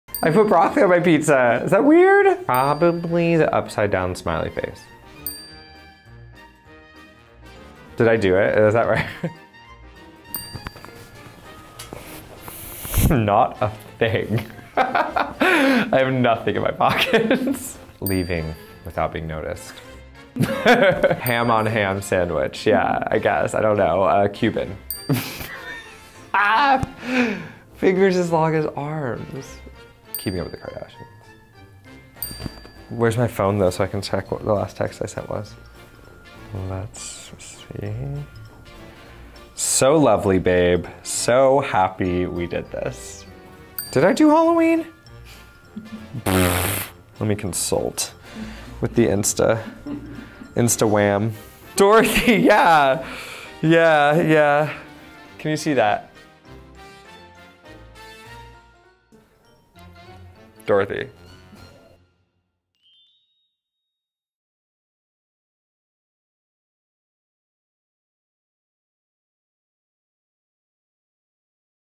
13 Reasons Why Star Tommy Dorfman Goes Speed Dating! | MTV Movies
Tommy Dorfman, who plays Ryan in pop culture phenomenon 13 Reasons Why, pops by for a speed date! And he’s here to share with you some AMAZING photos of his most recent fancy dress costume…